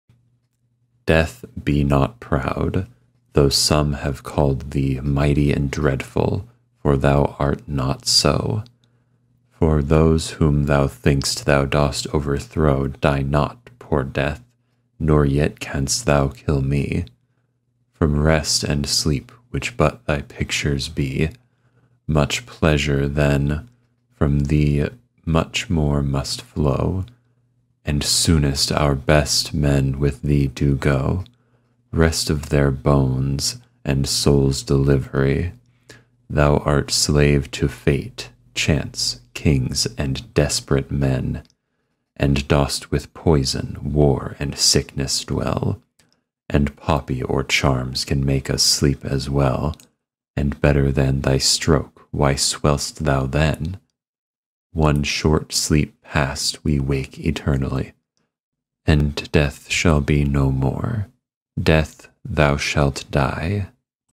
"Death, be not proud" by John Donne
I'm having to take a poetry class, this is one of them I'm working with. The screenshot is from the link at the bottom there, I'm the one that read it. Just figured I'd put this out there in case someone else was looking for a reading of the thing lol.